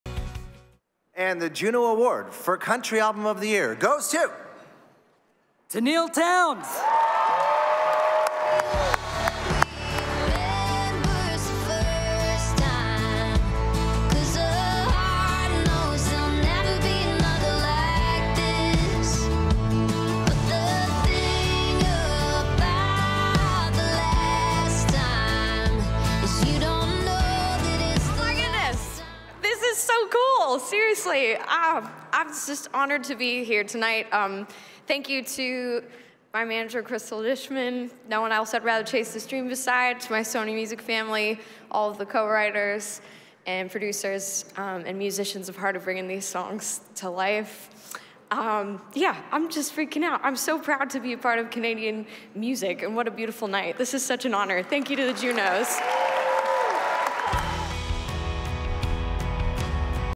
Tenille Townes wins Country Album of the Year | 2023 JUNO Opening Night Awards
@TenilleTownes wins Country Album of the Year Presented by Edmonton's Best Hotels at The 2023 JUNO Opening Night Awards Presented by Music Canada

Stay in the loop! Follow @TheJUNOAwards everywhere!